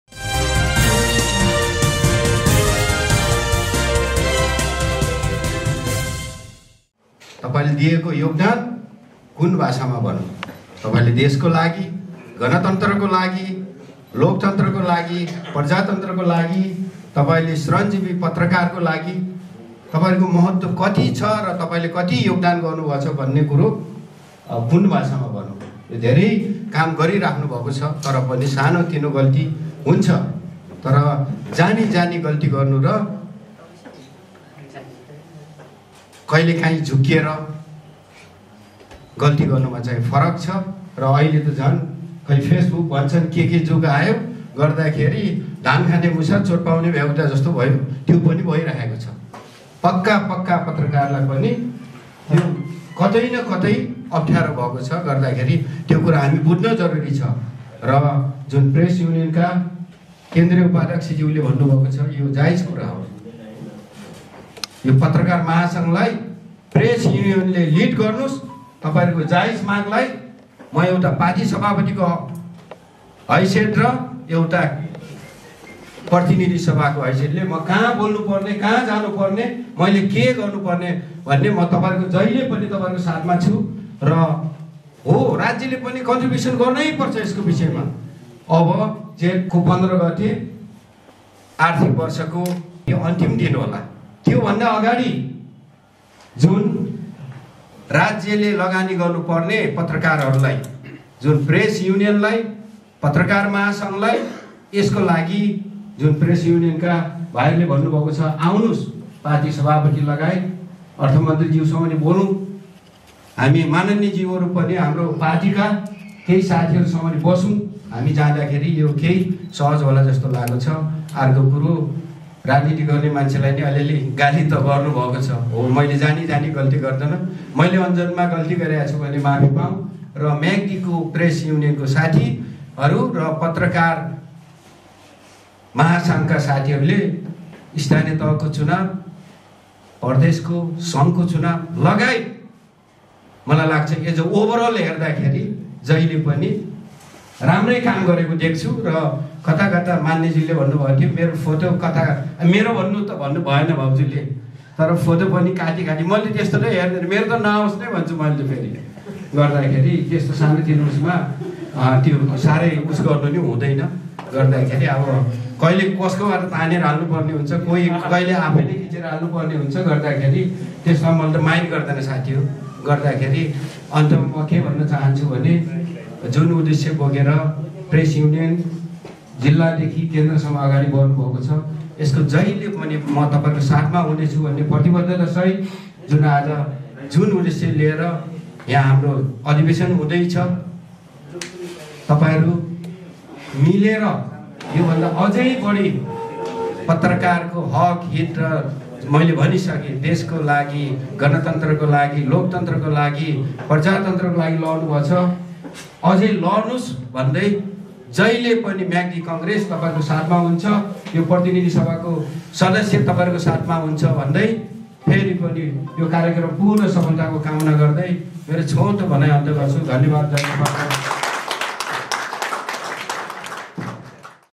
प्रेस जगत बलियो भयो भने मात्र लोकतन्त्र बलियो र संस्थागत हुन्छ : सांसद गर्वुजा || Myagdi News
प्नेपाल प्रेस युनियन जिल्ला शाखा म्याग्दीको आठौं साधारण सभा तथा छैठौं अधिवेशन कार्यक्रमको उद्घाटन गर्दै प्रतिनिधि सभा सदस्य खमबहादुर गर्वुजा ‘खम्बिर’ले मूलुकको ब्यवस्था परिवर्तनको क्षेत्रमा पत्रकारिता क्षेत्रले ठुलो योगदान दिएको बताउदै समाज परिवर्तन र जनजिविकाको अवस्था सुधार्न पत्रकारिता केन्द्रित हुनुपर्ने जोड दिनुभयो ।